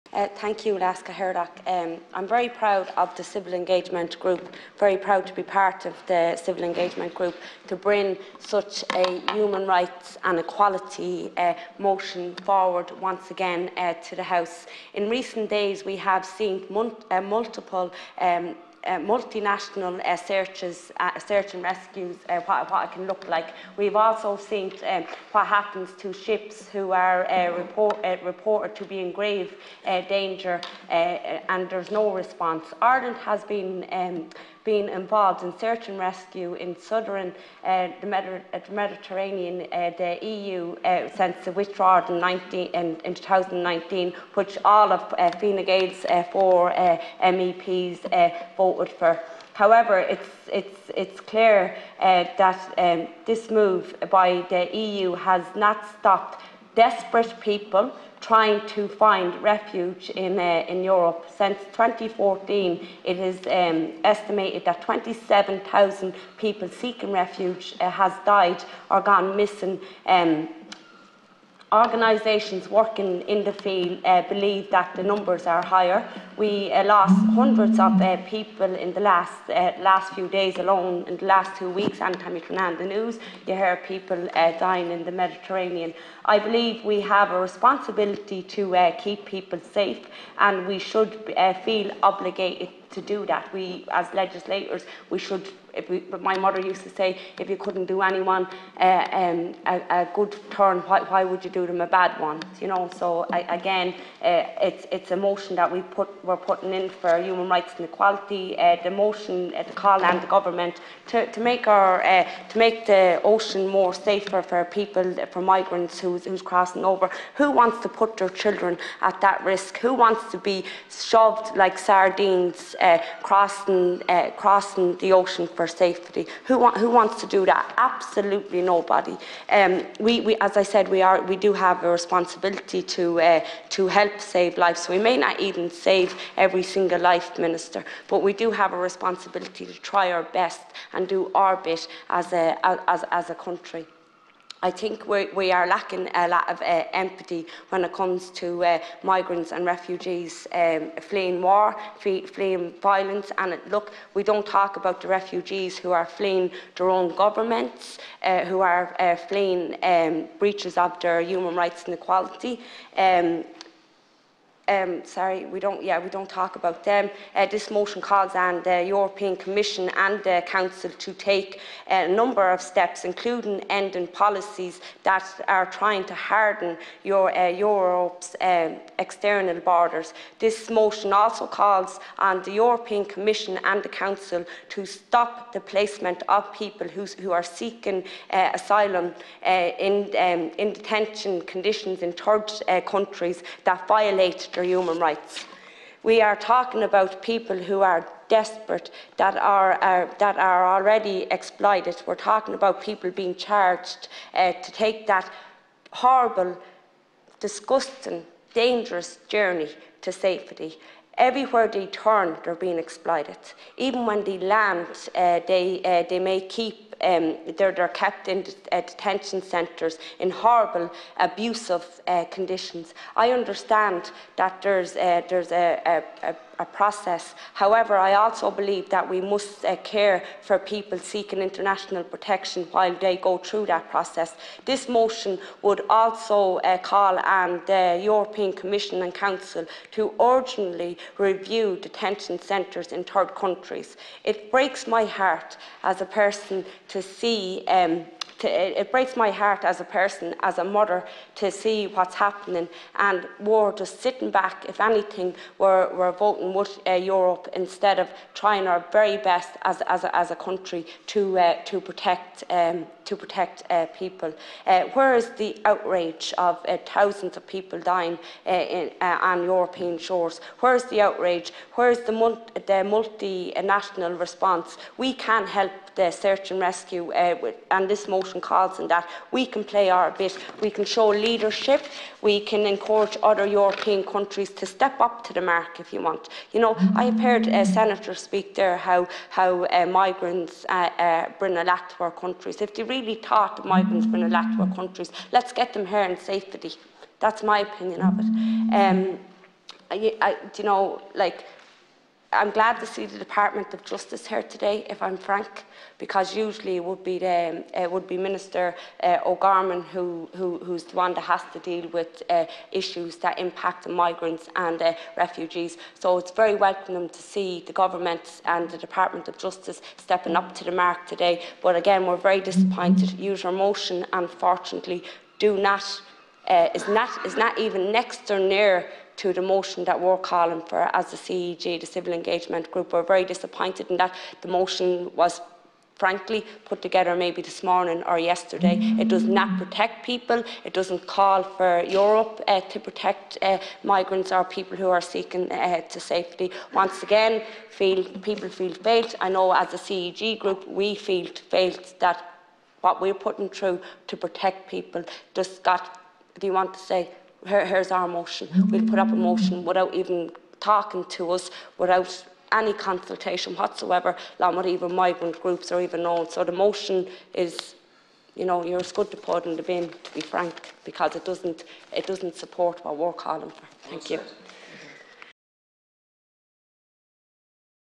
Senator Eileen Flynn- speech from 27 Jun 2023
Private Members' Business: Motion regarding Migration into the EU